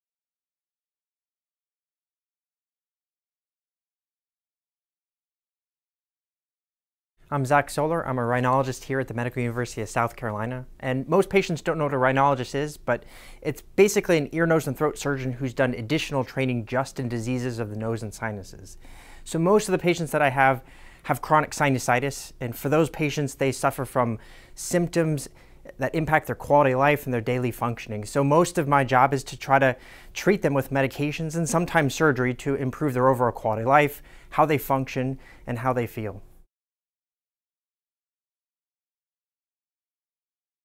Dr. Zach Soler, ENT - Rhinology & Sinus Surgery - MUSC Health
Zachary M. Soler, M.D., MSc, joined the MUSC sinus center in 2011. Originally from coastal Florida, Dr. Soler attended medical school at Wake Forest University, followed by a residency in otolaryngology -- head and neck surgery at Oregon Health and Science University. He then completed a fellowship dedicated solely to rhinology and endoscopic sinus surgery at Harvard Medical School. After fellowship, Dr. Soler spent an additional year at the Harvard School of Public Health, earning a Master’s Degree in epidemiology.

Dr. Soler’s practice is dedicated primarily to diseases of the nose, sinuses, and skull base. He treats adults and children with a wide range of conditions, ranging from common allergies and sinusitis to skull base tumors and CSF leaks. He has particular expertise with difficult-to-manage cases of sinusitis, revision sinus surgery, and surgical treatment of sino-nasal tumors. He sees patients in Mount Pleasant, North Charleston, and downtown Charleston.

Dr. Soler has authored over 100 manuscripts and book chapters related to otolaryngology and rhinology. His research focus is on optimizing clinical outcomes after medical and surgical treatment of chronic sinusitis. He is a principal investigator on several large studies funded through the National Institutes of Health and the American Rhinologic Society. He is board certified through the American Board of Otolaryngology and a member of the American Rhinologic Society.